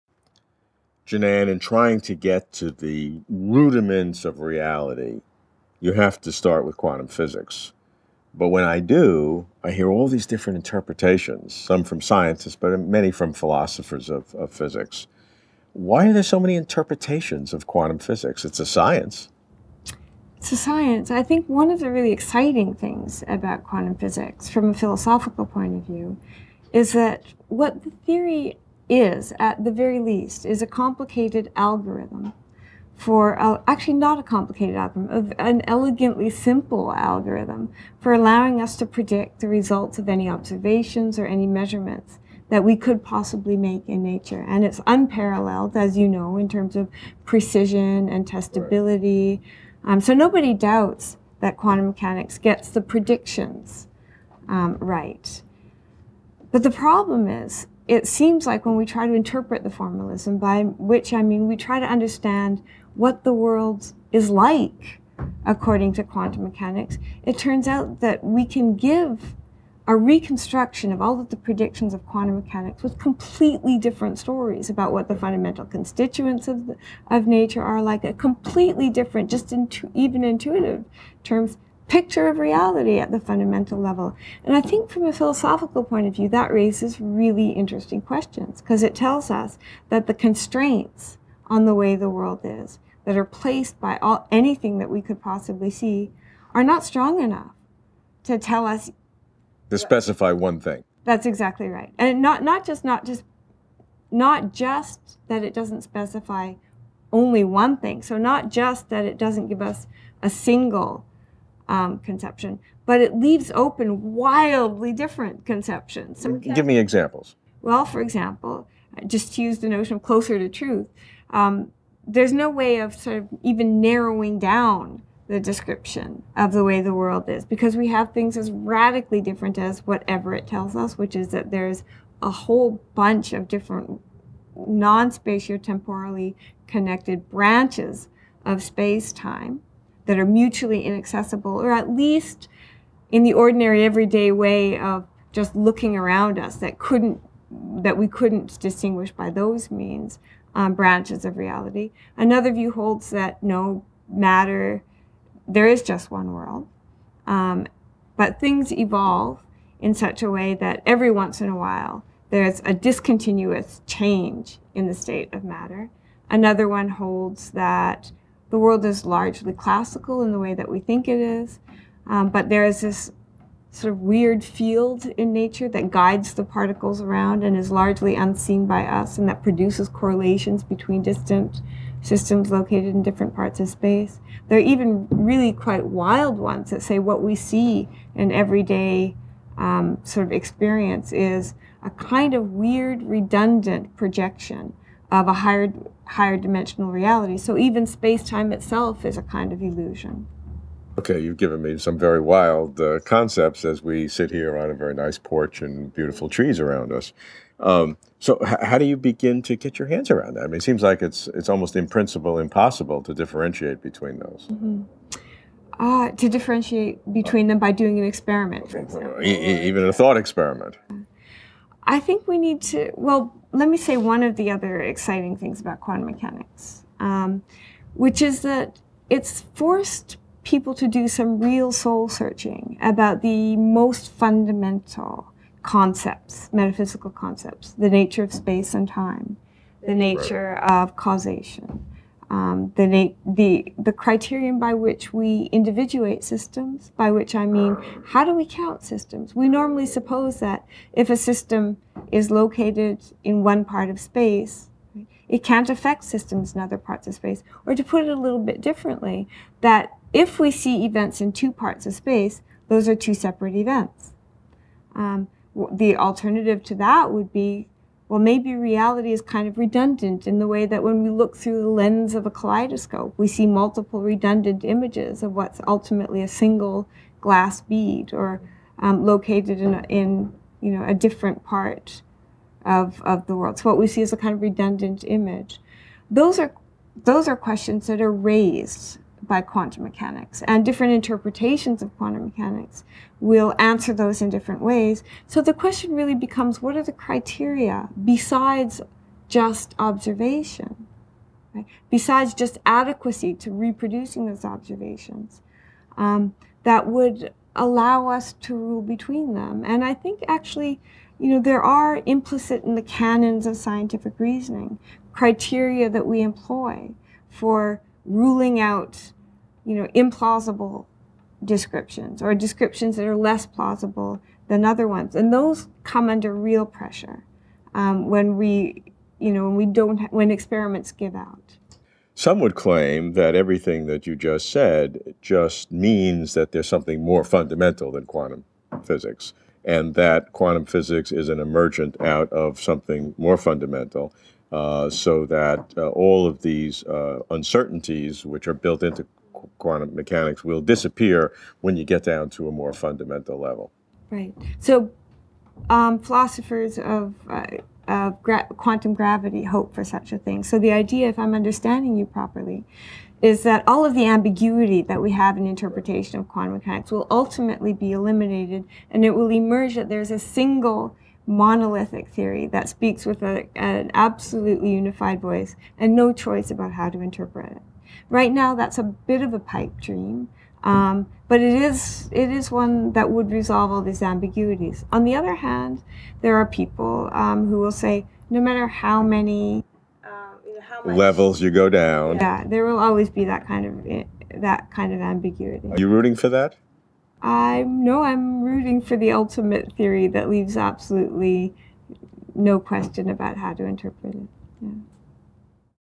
Jennan Ismael - What Does Quantum Theory Mean?
Quantum theory may be weird—superposition and entanglement of particles that in our normal world would make no sense—but quantum theory is truly how the microworld works. What does all this weirdness mean? How to go from microworld weirdness to macroworld normalcy? Will we ever make sense out of quantum mechanics?

Jenann Ismael is an Associate Professor of Philosophy at the University of Arizona and a member of the Foundational Questions Institute (FQXi.)